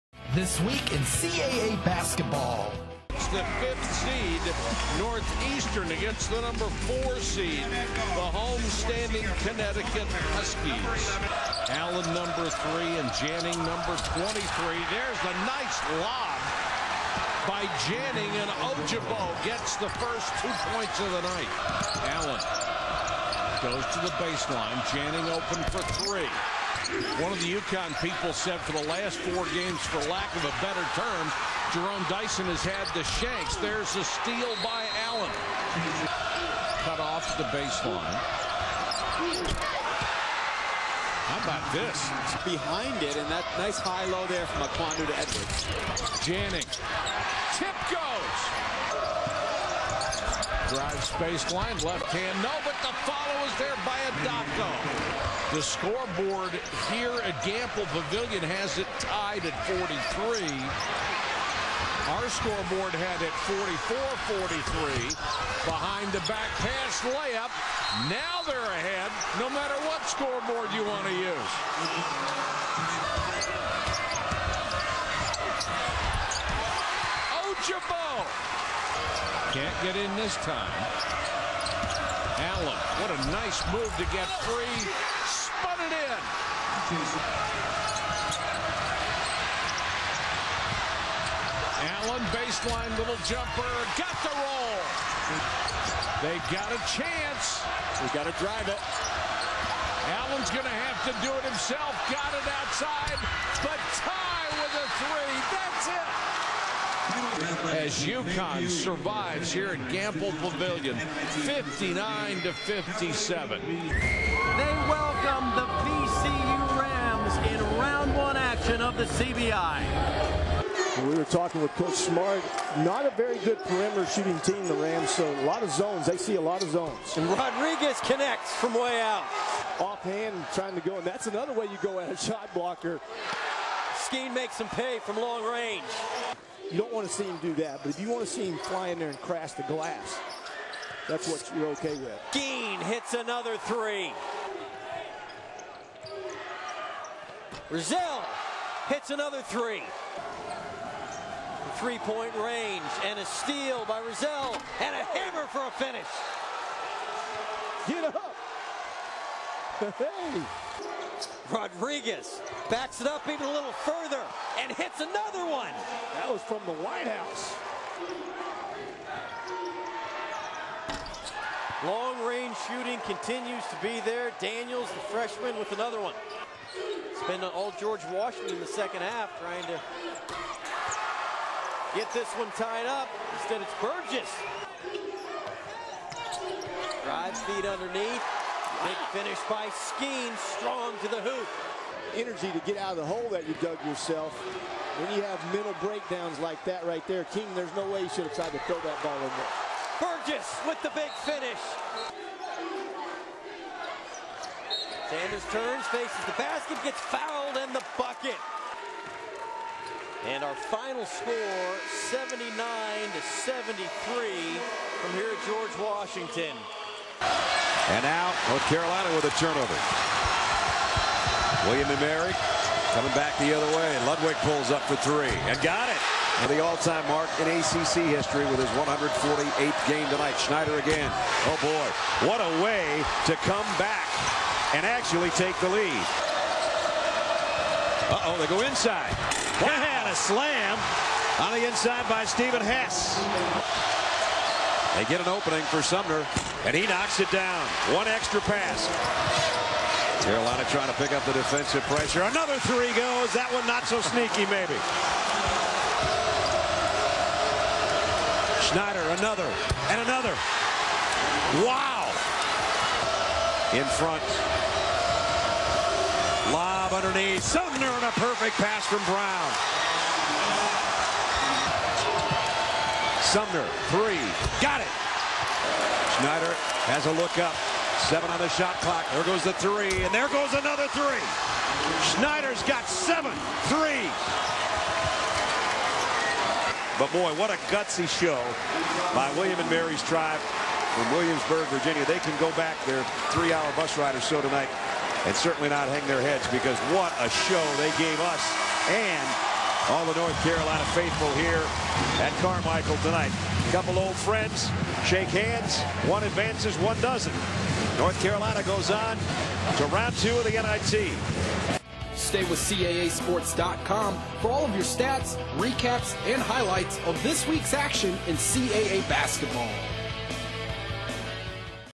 CAA Men's Basketball Post-Season Recap - March 16th, 2010
CAA Men's Basketball Post-Season Recap - March 16th, 2010; NIT 1st Round Northeastern @ Connecticut; CBI 1st Round VCU @ George Washington; NIT 1st Round William & Mary @ North Carolina